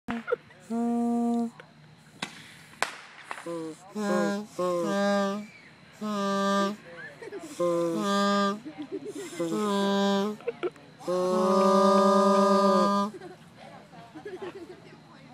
Zucchini flutes
Darren and Emily playing the zucchini flutes at Emily's 4th of July BBQ.